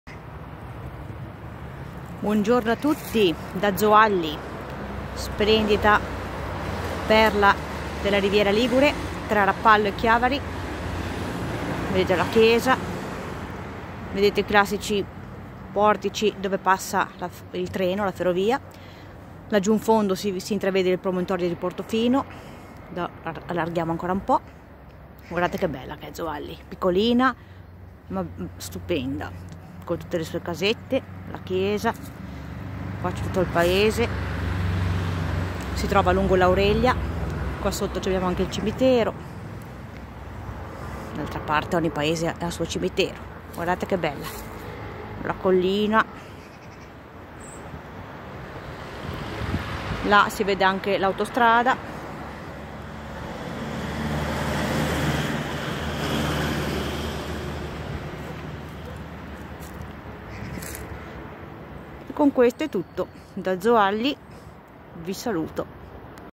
Zoagli, piccola perla del Tigullio
tra Rapallo e Chiavari ecco un piccolo paese grazioso di nome Zoagli